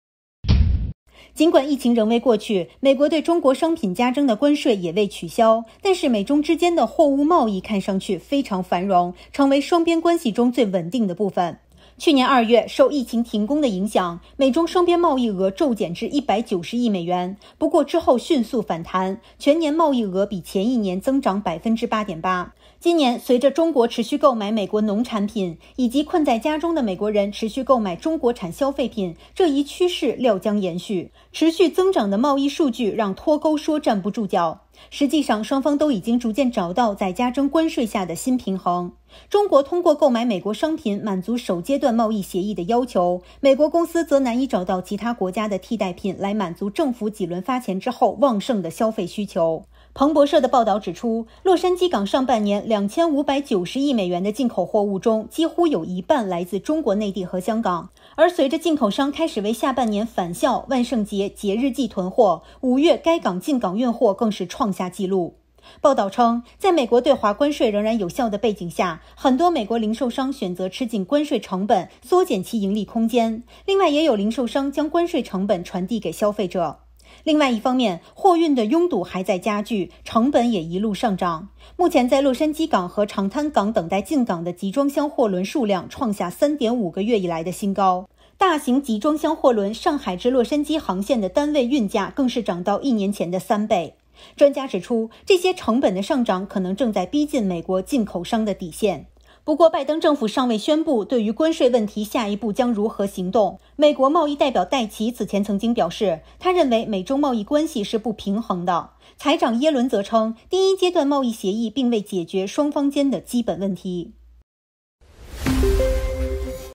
疫情、贸易战导致脱钩？美中贸易额屡创新高
尽管疫情仍未过去，美国对中国商品加征的关税也未取消，但美中之间的货物贸易看上去非常繁荣，成为双边关系中最稳定的部分。
去年2月，受疫情停工影响，美中双边贸易额骤减至190亿美元，不过之后迅速反弹，全年贸易额比前一年增长8.8%. 今年，随着中国持续购买美国农产品，以及困在家中的美国人持续购买中国产消费品，这一趋势料将延续。
持续增长的贸易数据让“脱钩说”站不住脚，实际上，双方都已逐渐找到在加征关税下的新平衡。中国通过购买美国商品满足首阶段贸易协议的要求；美国公司则难以找到其他国家的替代品，来满足政府几轮发钱后旺盛的消费需求。